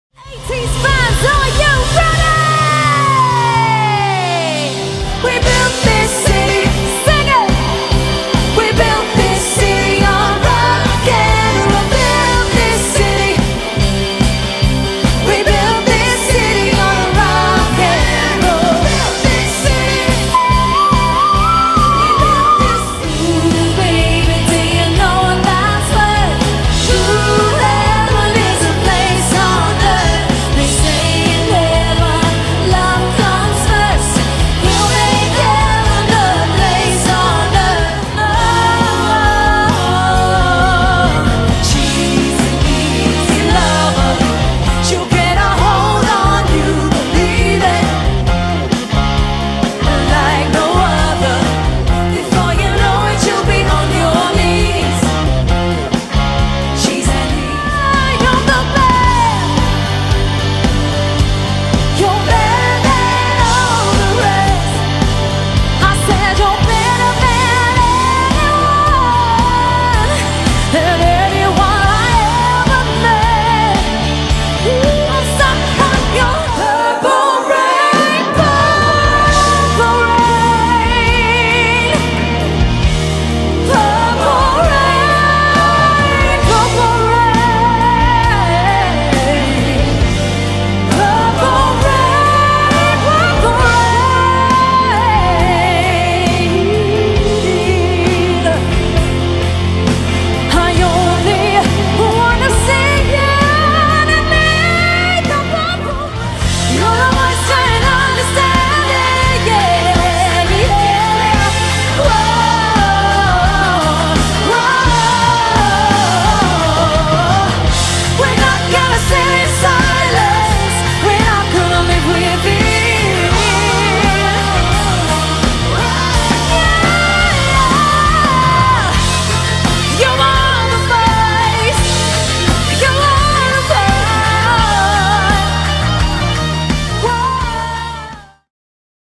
Virtual 80s Mix Tape - AliveNetwork.com
Taking your online event back in time, Virtual 80's Mix Tape bring you everything 80’s performed with energy, passion & epic hits! Prepare to have your guests singing along and air-guitaring into the night!